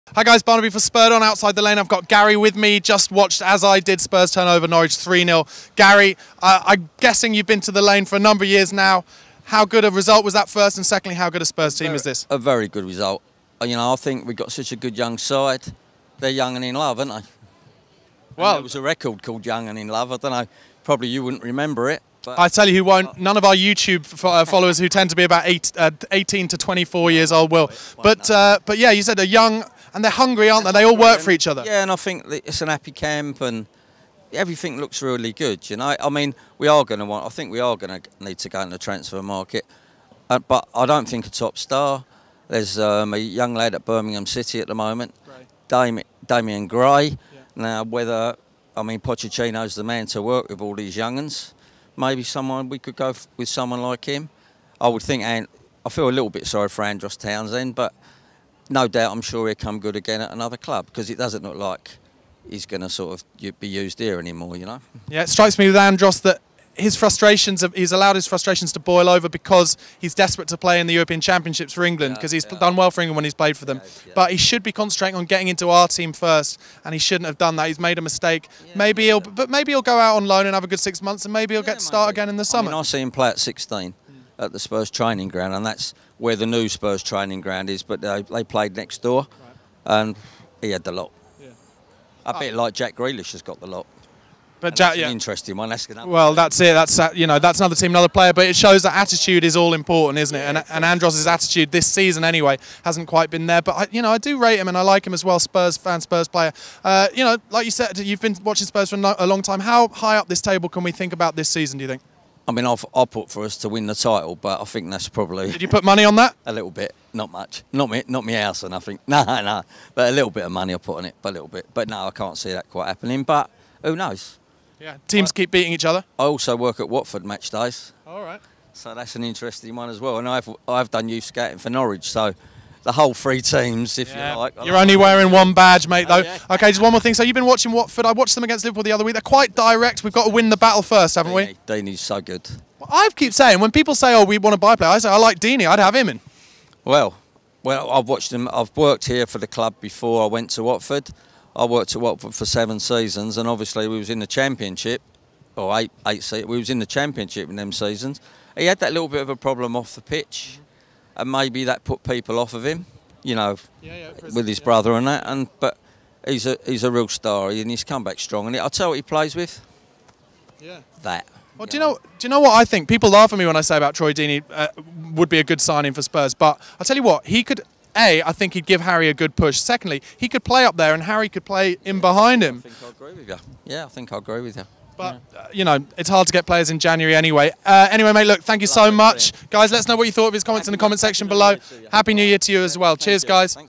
Tottenham Hotspur 3-0 Norwich | "We've Got a Great Young Side!" | Fan Cam
This fan talked about our great young side after the Spurs 3-0 Southampton game.

Facebook.com/coysnews